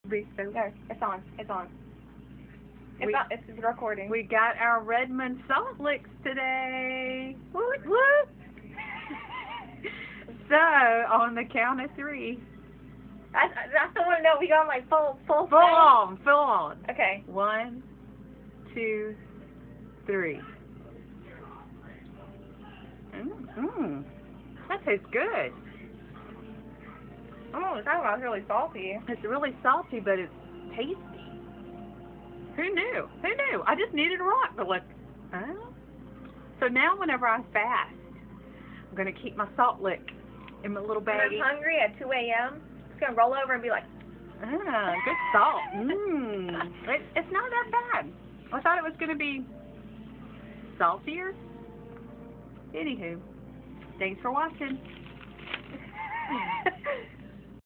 Got my Redmond's Bundle. Trying the salt rock.
My daughter and I are trying the salt rocks.